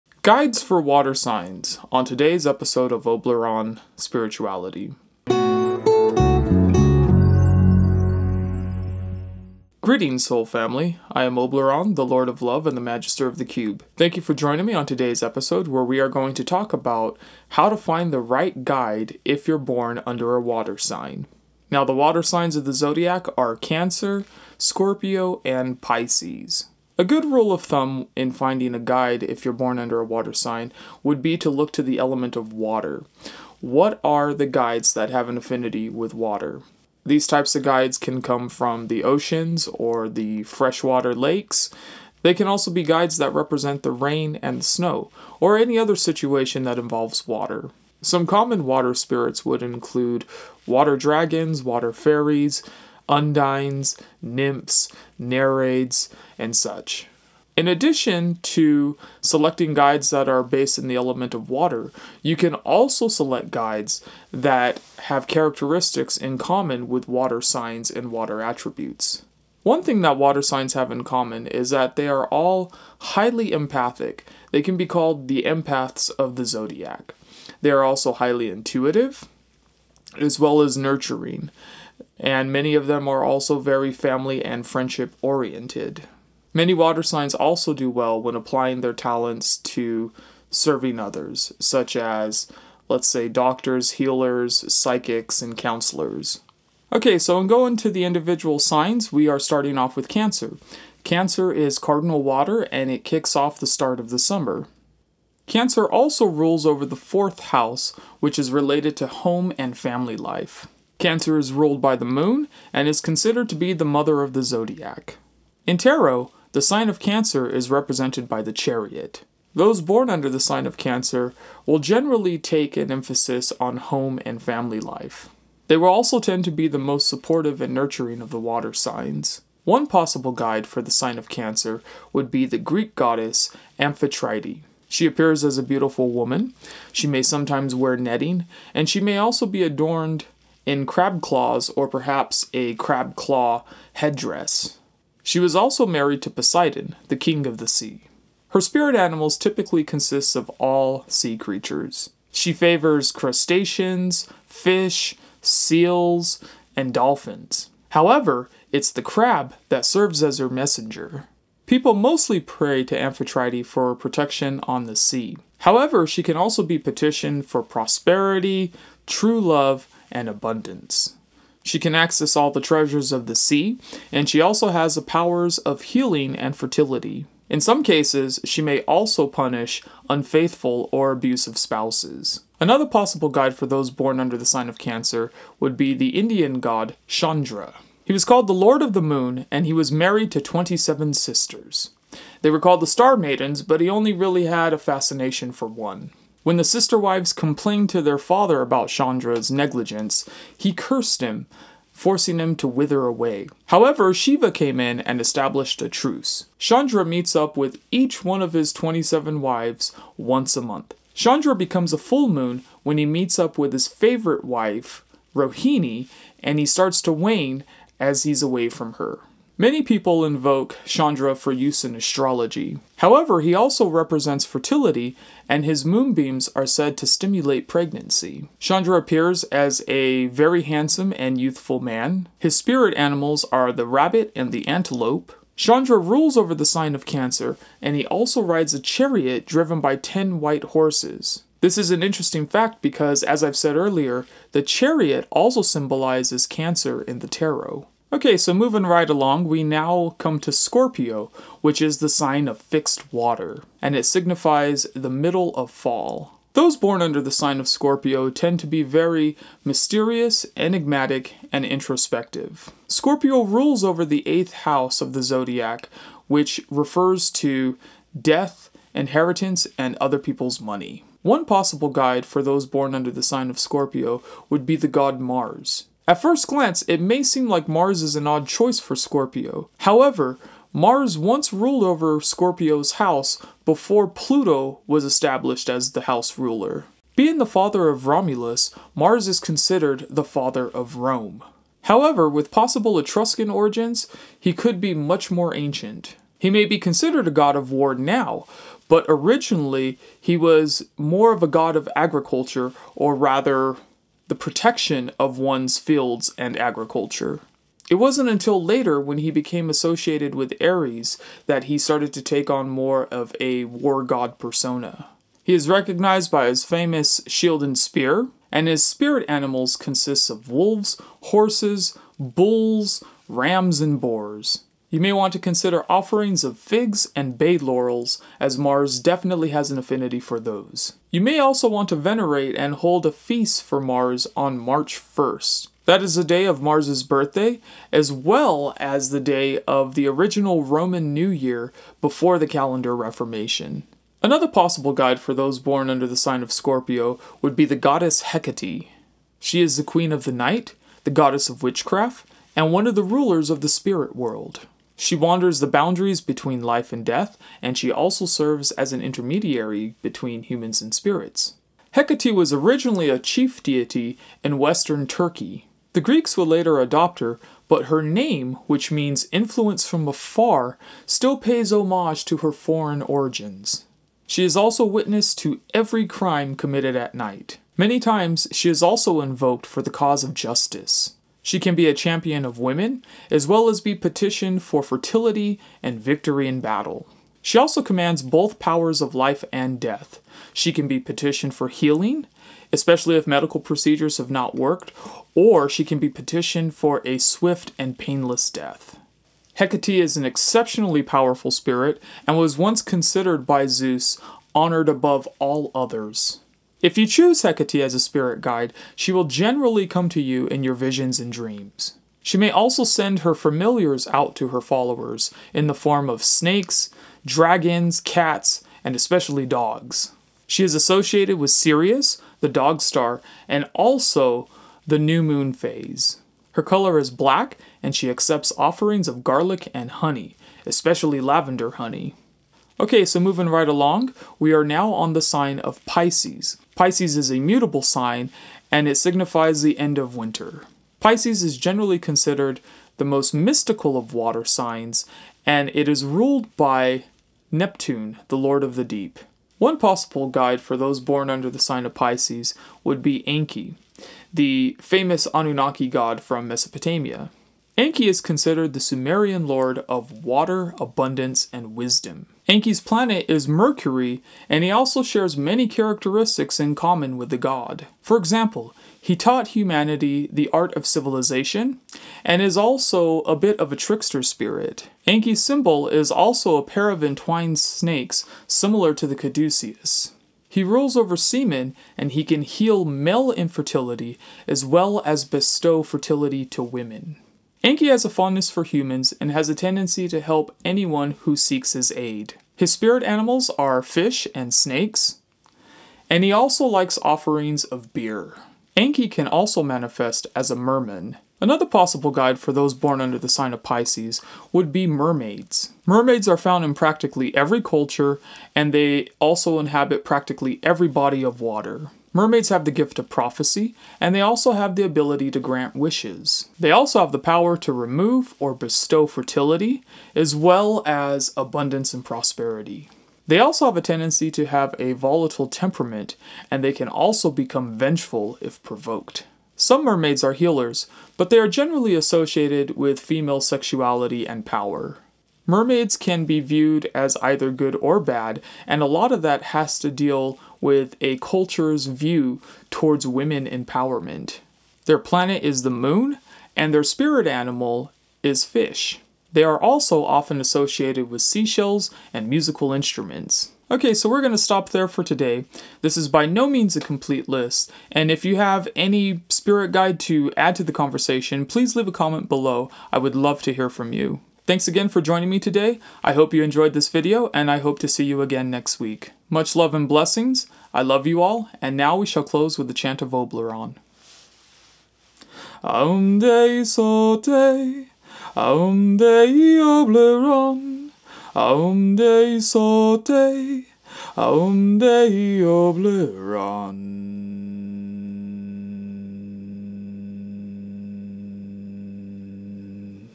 Water Signs, Your Guides Are Waiting For You - Cancer, Scorpio, Pisces - Spirit Guides
Oblyron discusses the various guides that work well for those born under one of the three water signs Cancer, Scorpio and Pisces.

Reference Book:

Encyclopedia of Spirits by Judika Illes